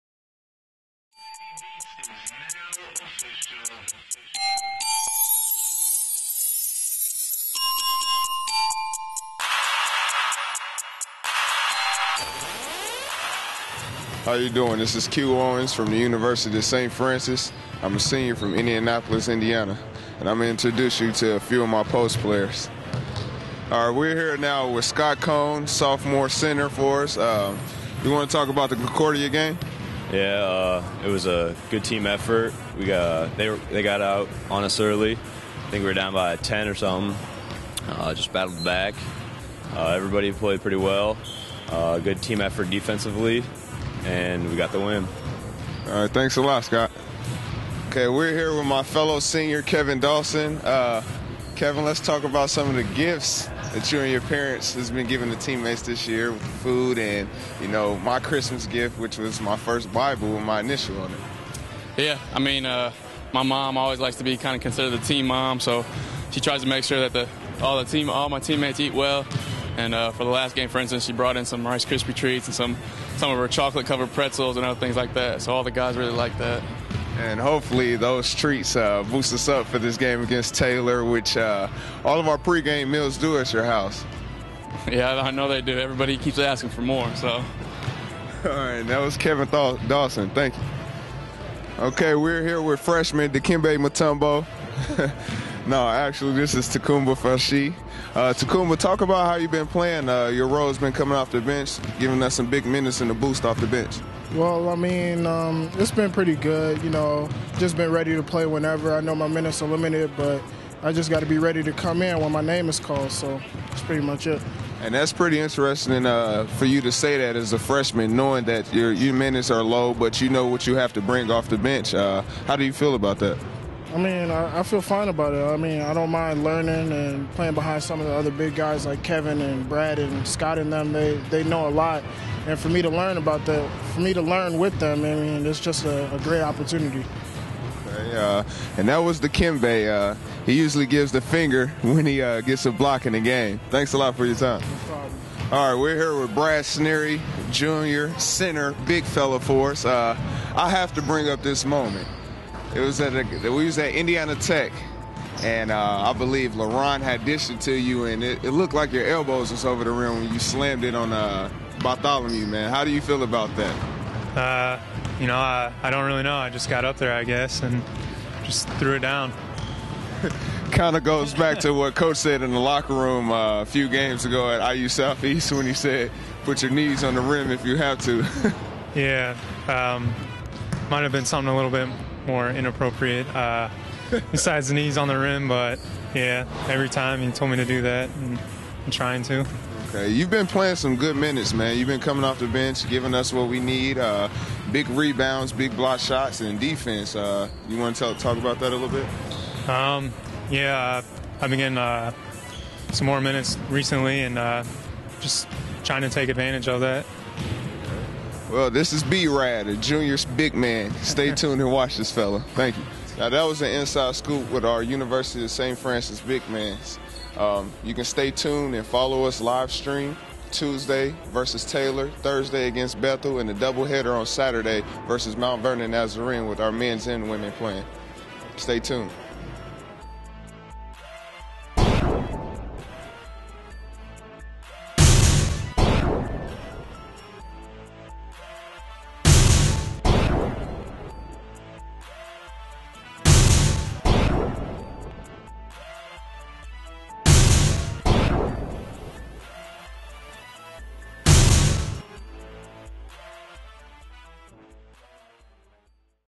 1-2-2012, Meet the Cougars Men's Basketball
University of Saint Francis senior Q Owens talks with Scott Kohne, Kevin Dawson, T.K. Fashawe and Brad Sneary. The men's basketball team next plays Taylor, 1/3/12, 7pm at Hutzell.